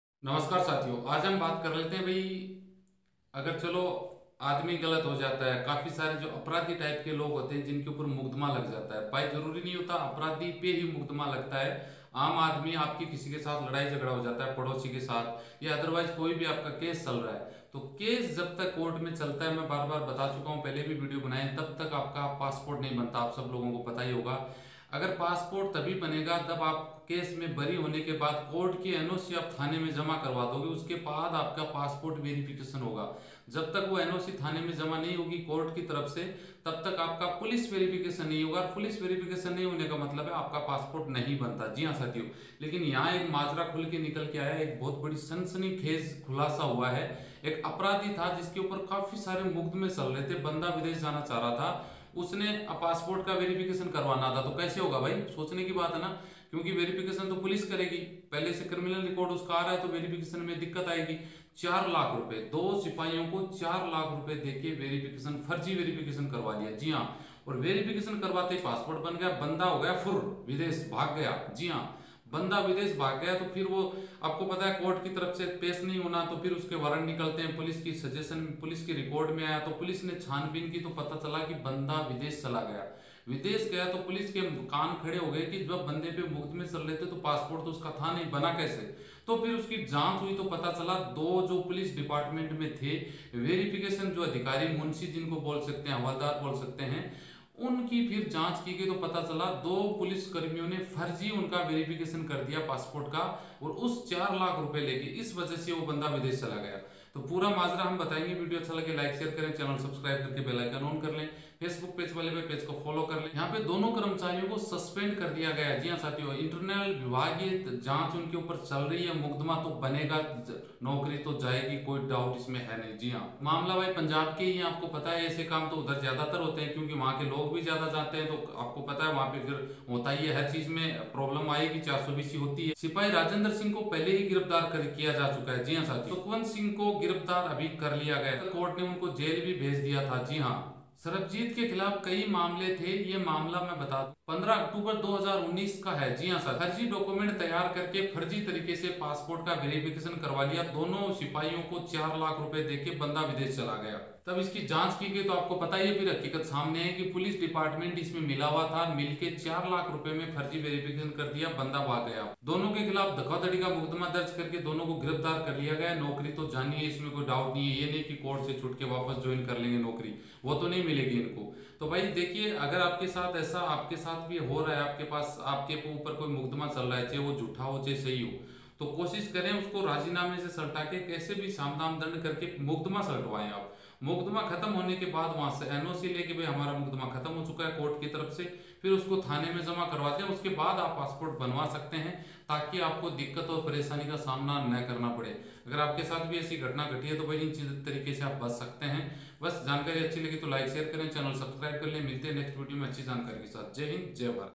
पासपोर्ट का फर्जी वेरीफिकेशन किया।The criminal who had fake verification of passport fled abroad.
The criminal who had fake verification of passport fled abroad.,
passport status check,
passport New making,
passport New charge,
passport verification,

Account-Ashok Kumar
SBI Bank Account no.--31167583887
IFSC Code-SBIN0031123